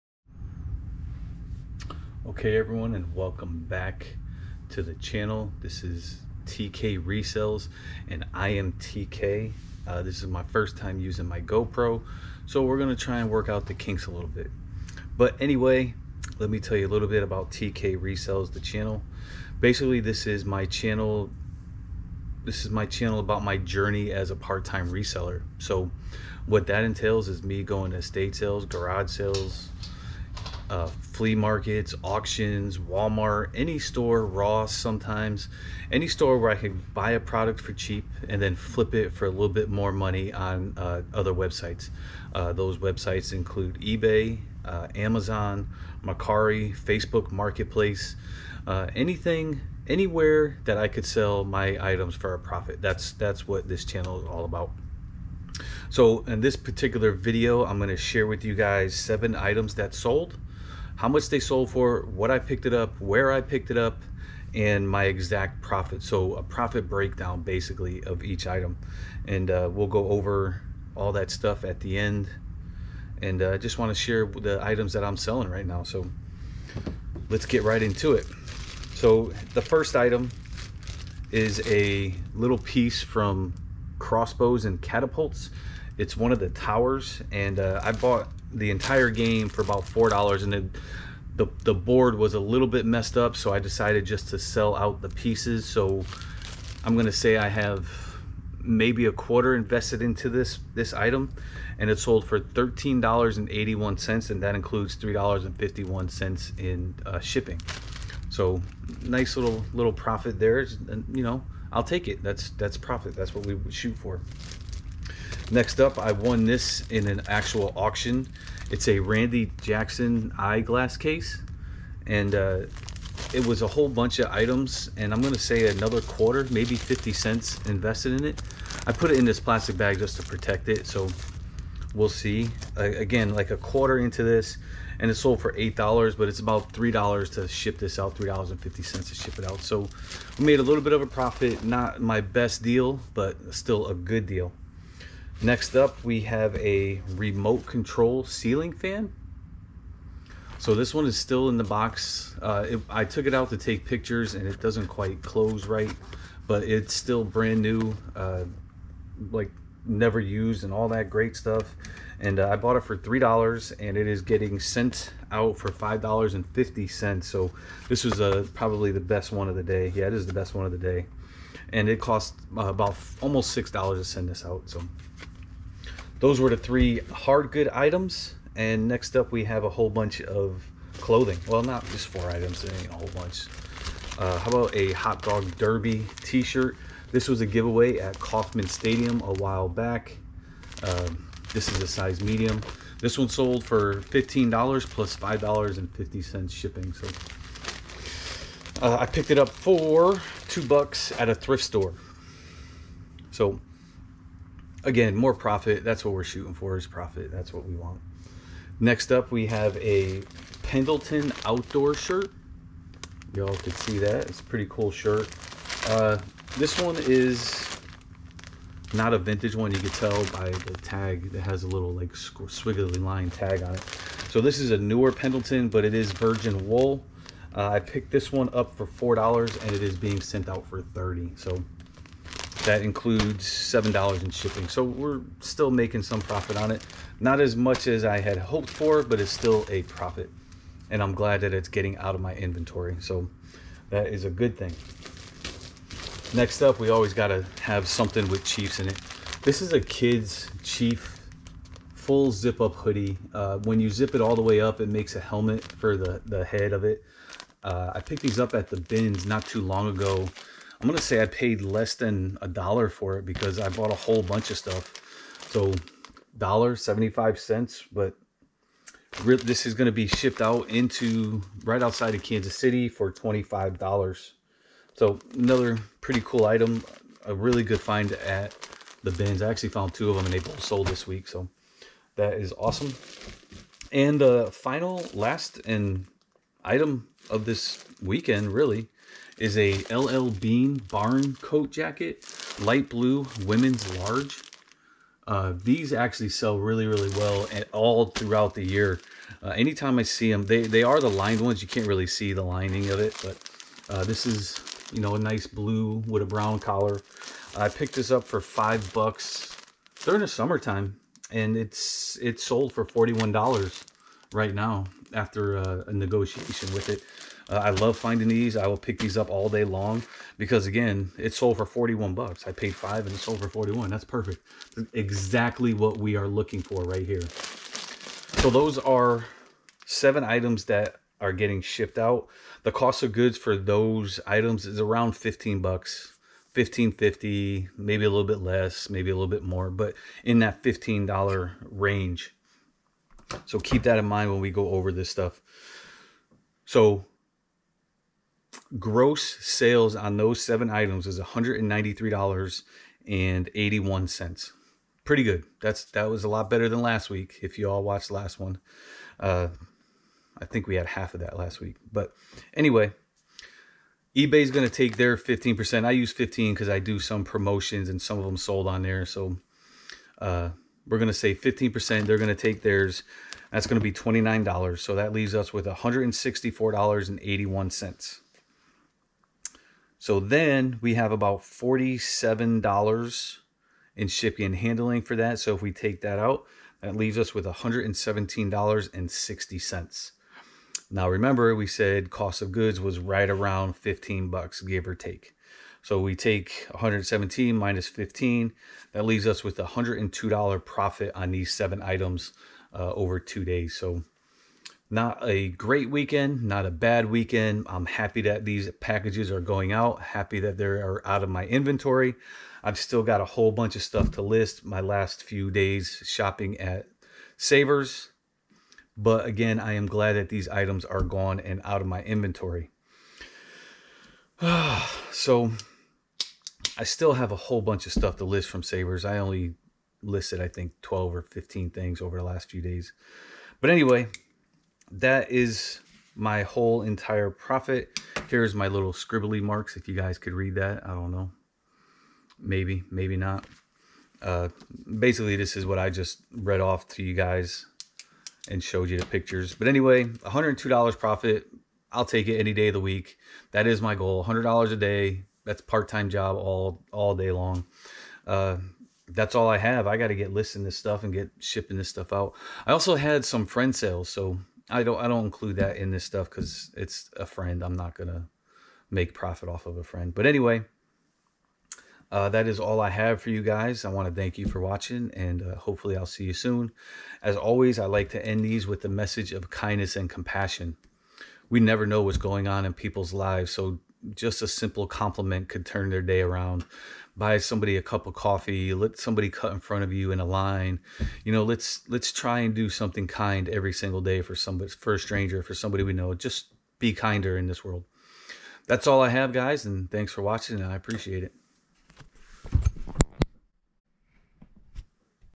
Castle, Remote Control, Barn Coat What sold and for how much profit! 2-1-21
TK Resells shares his journey as a part-time reseller.

In this video, TK Resells shares several items that sold plus he gives a detailed breakdown of the exact profit made.

Link to Ebay store